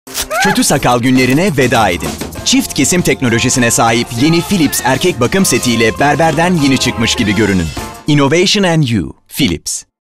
Yeni Philips Erkek Bakım Seti ile Tanışın!
Çift kesim teknolojisine sahip Yeni Philips Erkek Bakım Seti ile kötü sakal günlerine veda edin! Özel başlıklarıyla berberden yeni çıkmış gibi görüneceksiniz.